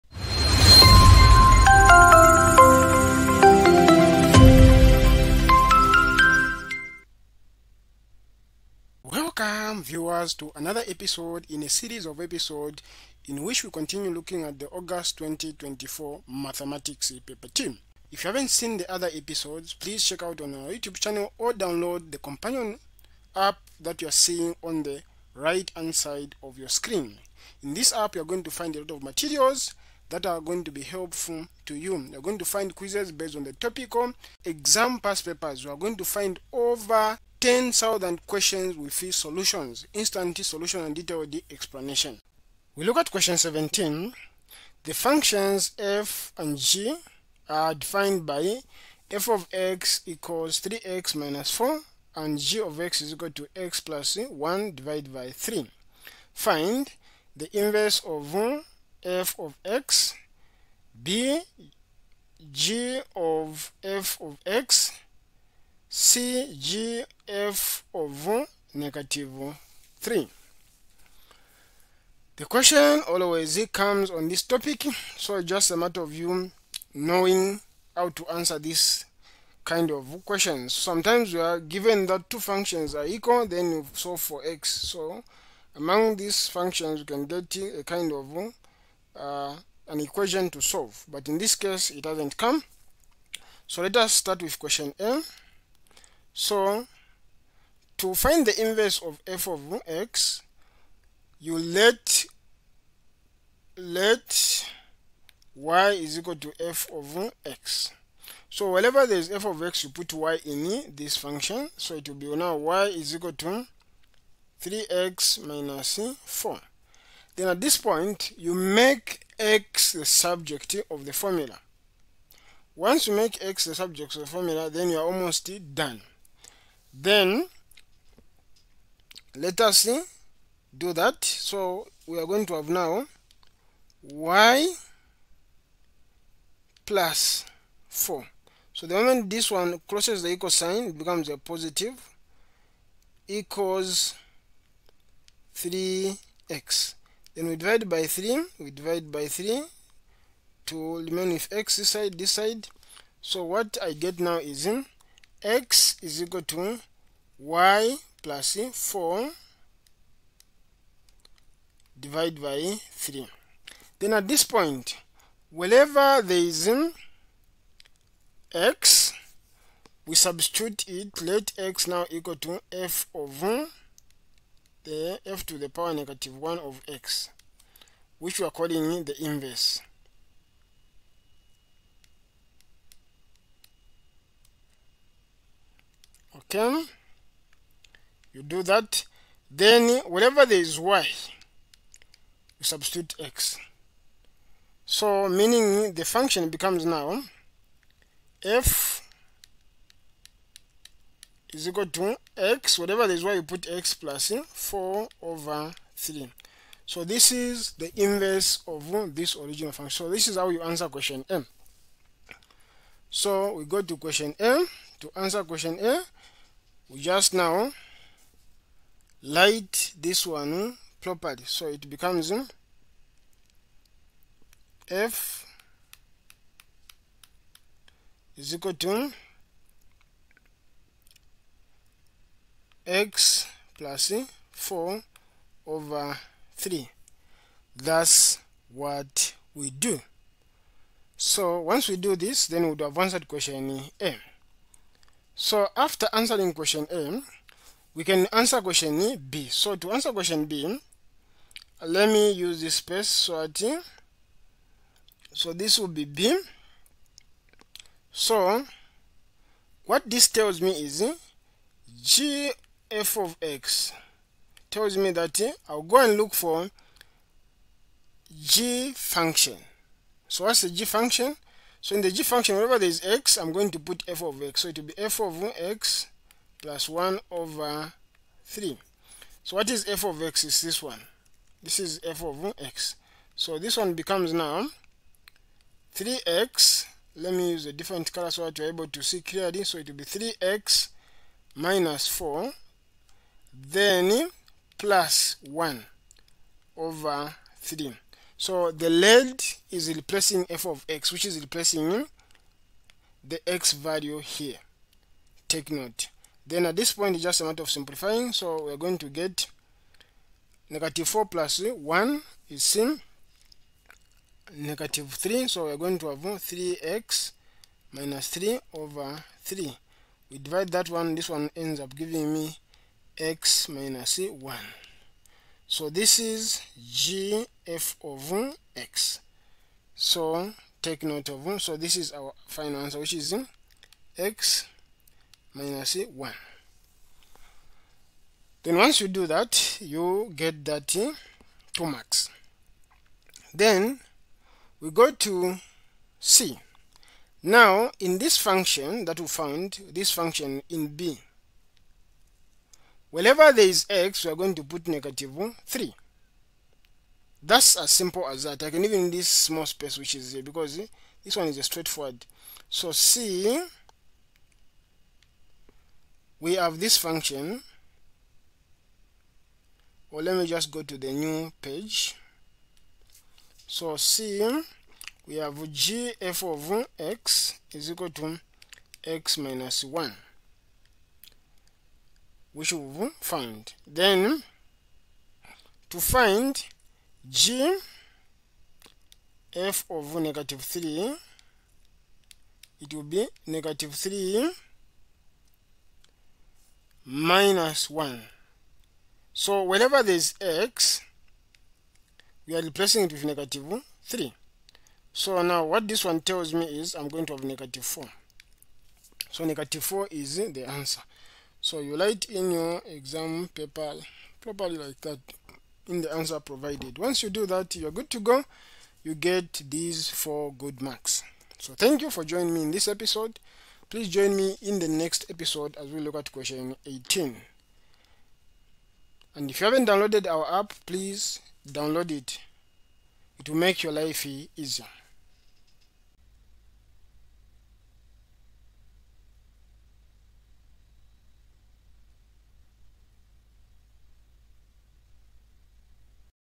2024 GCE MATHEMATICS PAPER 1 QUESTION 17
Are you struggling to understand the 2024 GCE Mathematics Paper 1 solutions? Look no further! In this video, we'll break down each question step-by-step, providing clear explanations and helpful tips.

Join our WhatsApp group for instant updates, get an the Study Companion, discussions, and support.